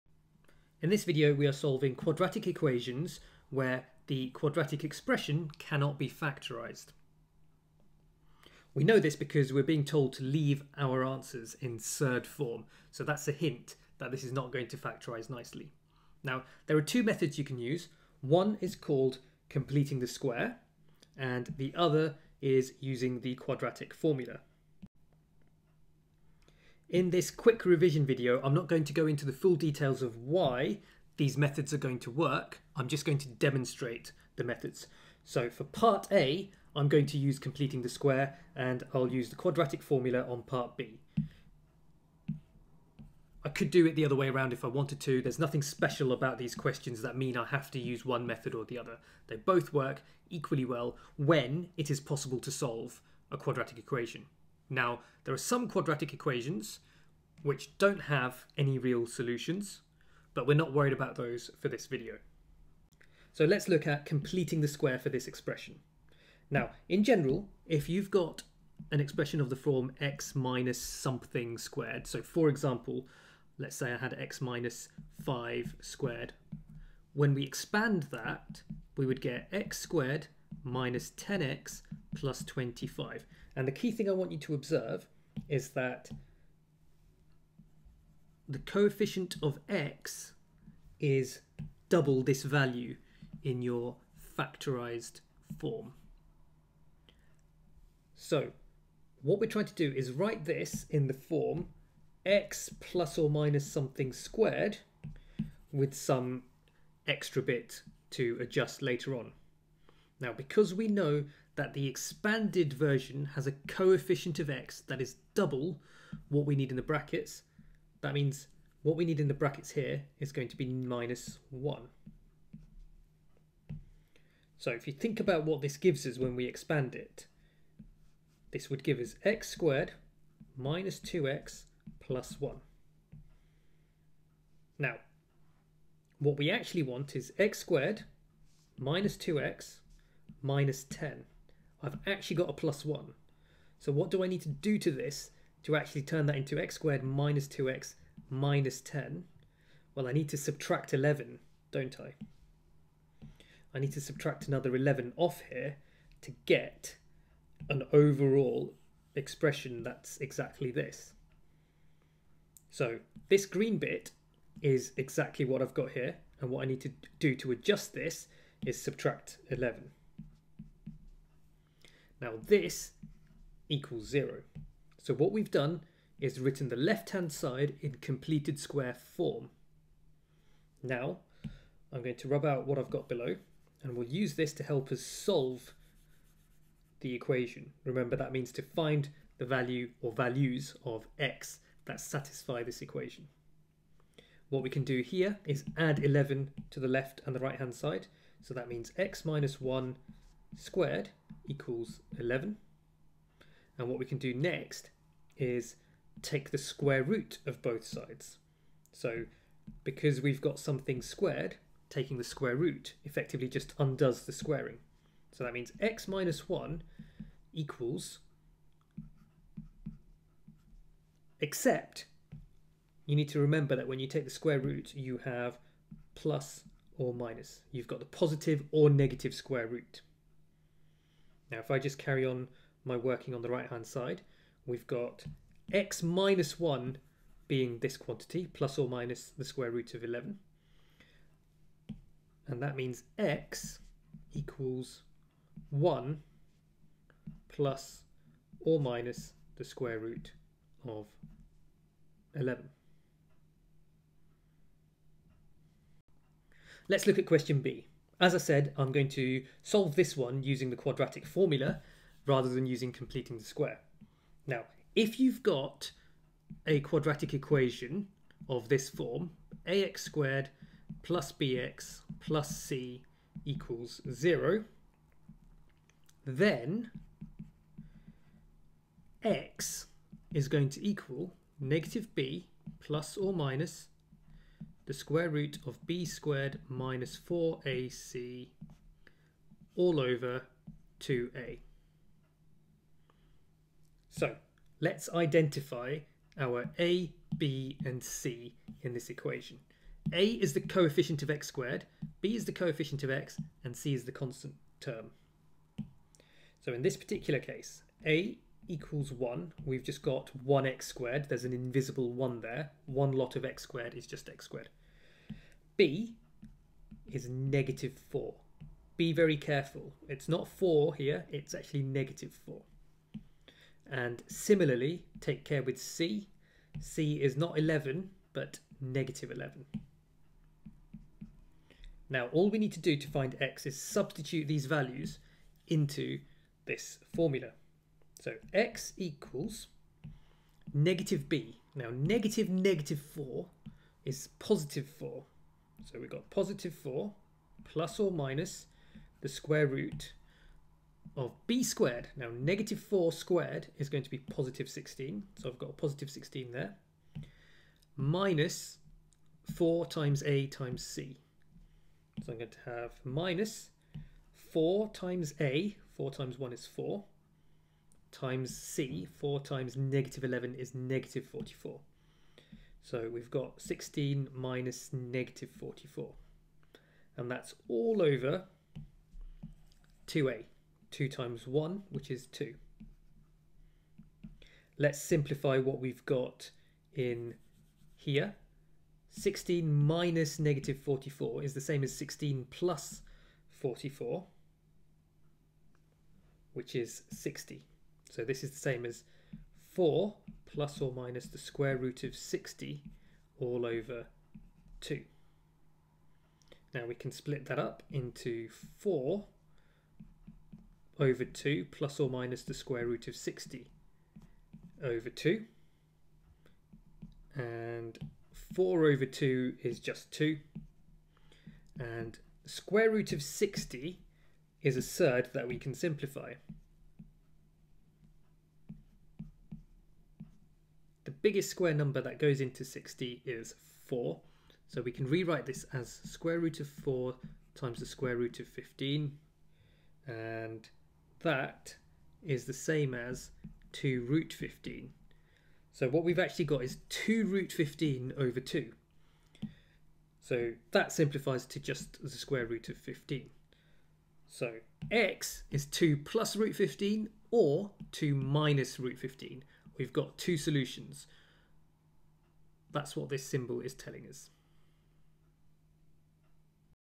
BossMaths Solving equations Quick Revision video 7 of 8
Solving quadratic equations by completing the square and by using the quadratic formula.